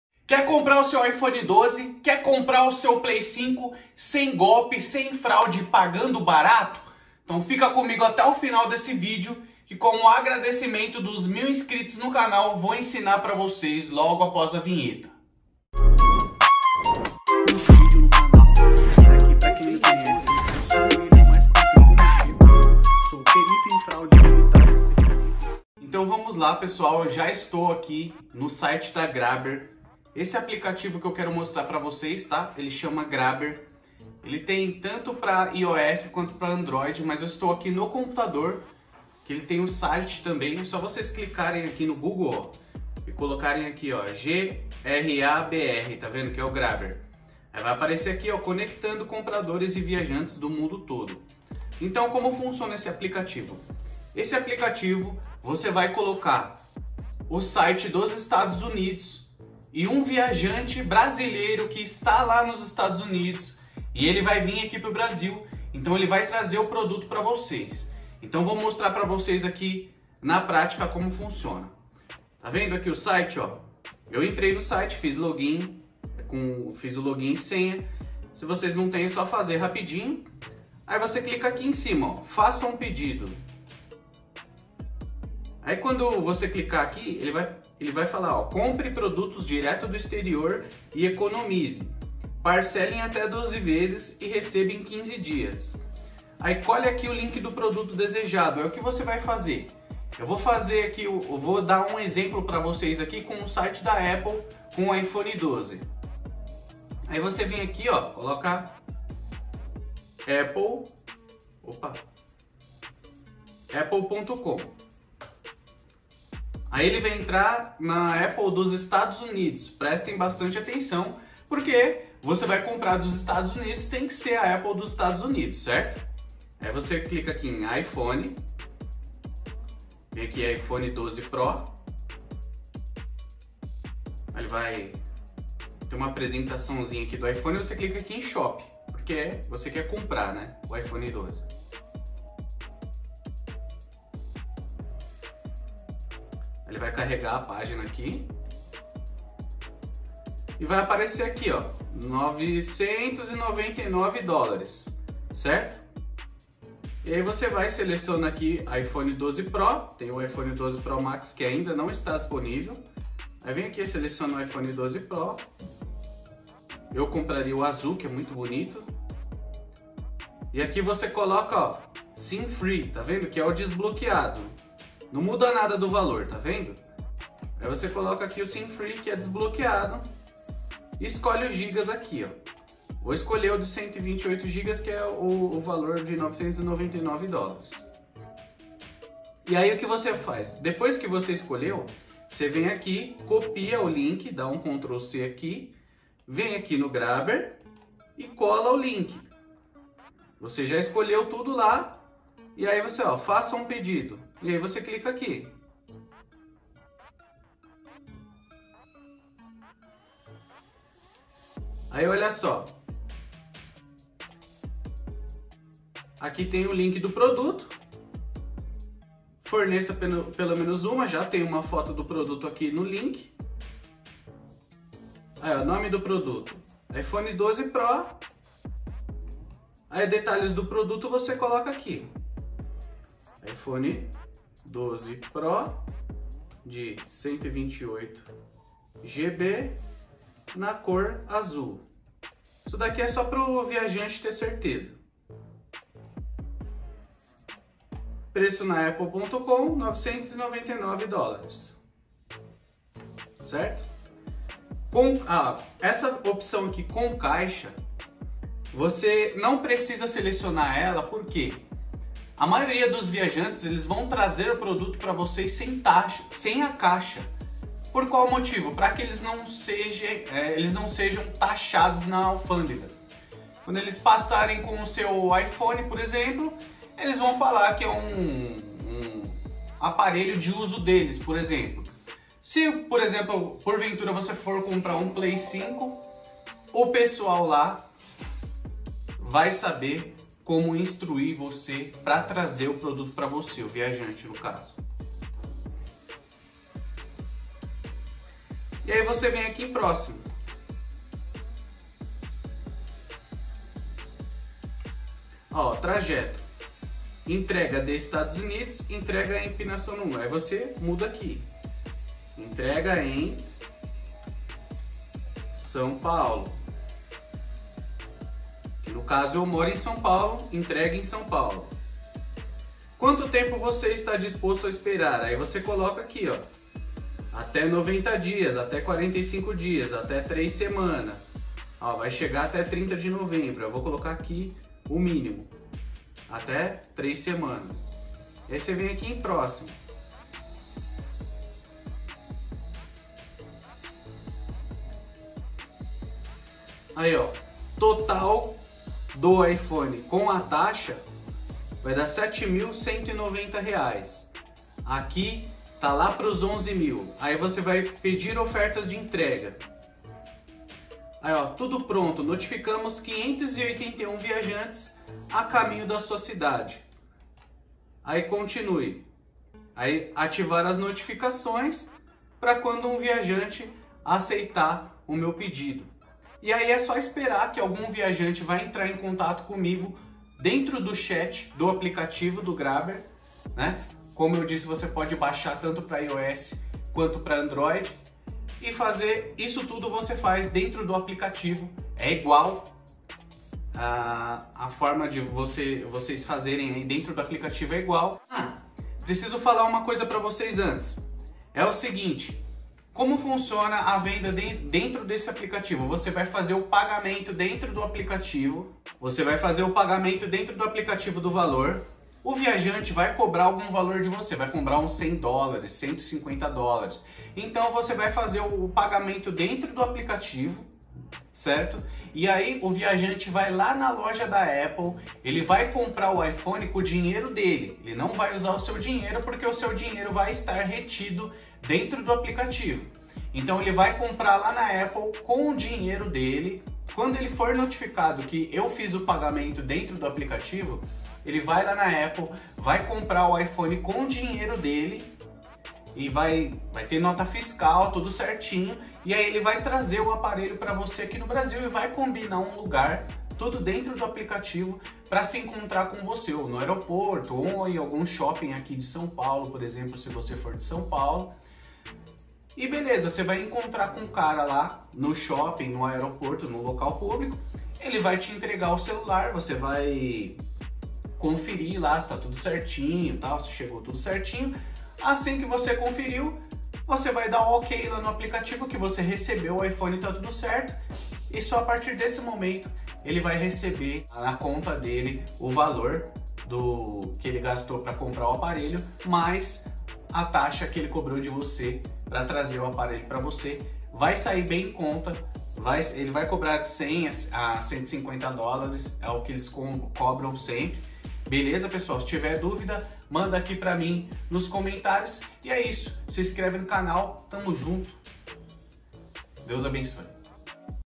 SEGREDO REVELADO - Como comprar Iphone 12 e Play 5 nos EUA com preço justo e sem golpe!
Fala Pessoal, nesse vídeo ensino vocês utilizarem o GRABR, app pra IOS e ANDROID que disponibiliza comprarmos dos Estados Unidos com o preço de lá e um viajante viaja até o Brasil para nos entregar o produto.

Assista e aprenda!

Loja de produtos Apple e Xiaomi - Informe que veio pelo @willoperito e ganhe desconto: